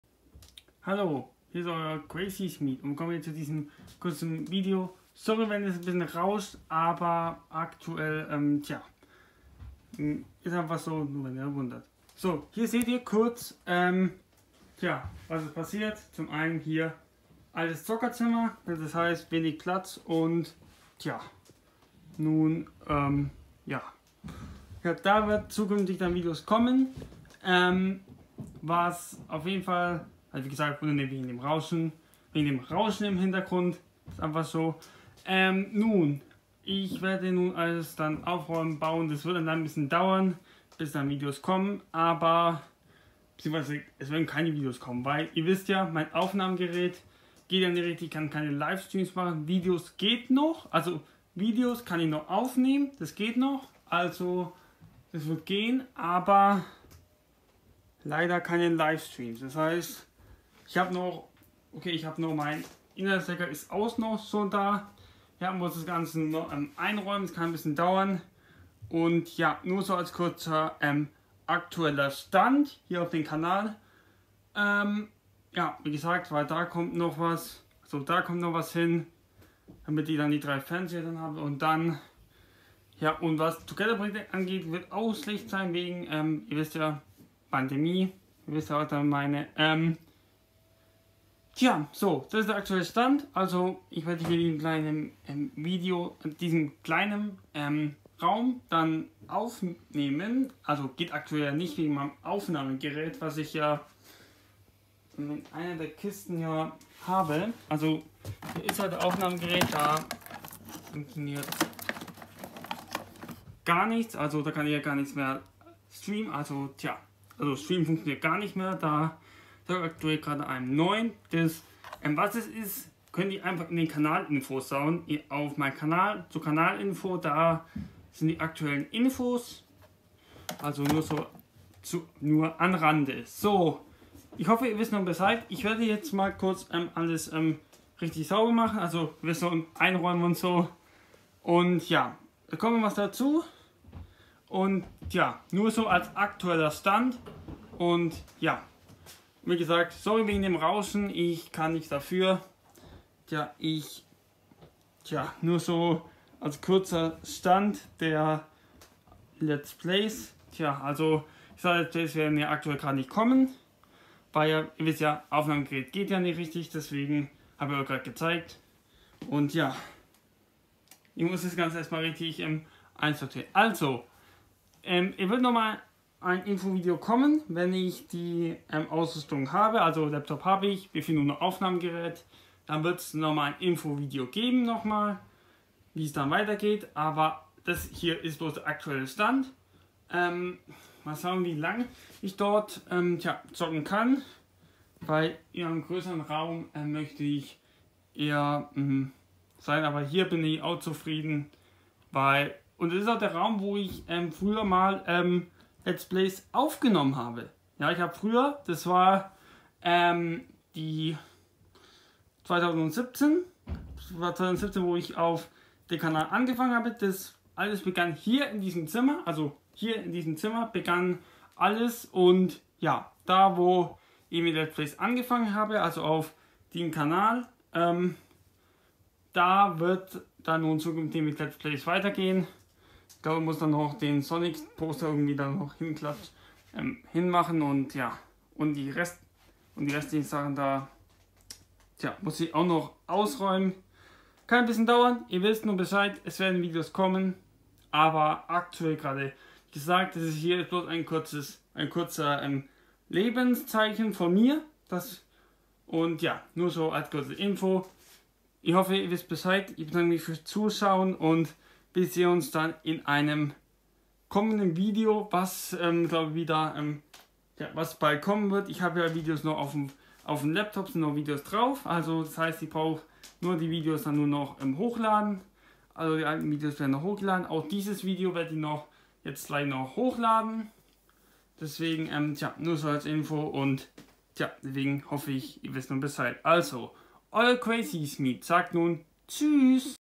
Ein kurzer Lebenszeichen von mir.
Hier nur als Info, das dieser Kanal nicht gestorben ist.